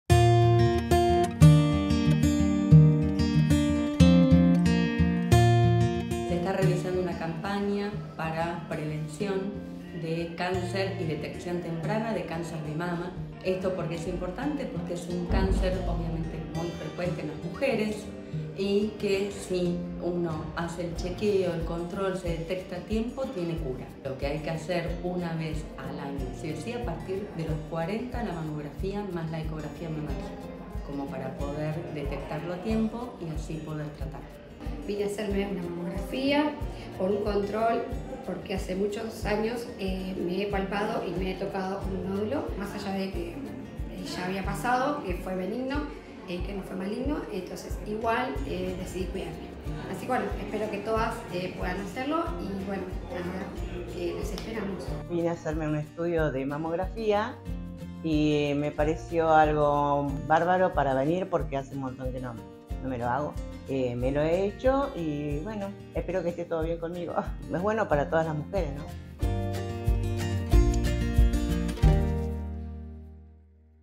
Tres días de jornada de mamografías gratuitas y sin turno en hospitales públicos de BAP
| Durante estos tres días de jornada de mamografías gratuitas y sin turno en hospitales públicos de @BAProvincia, realizamos más de 1700 estudios.
La detección temprana del cáncer de mama es fundamental para iniciar el tratamiento a tiempo y curar la enfermedad.

20 de octubre 2024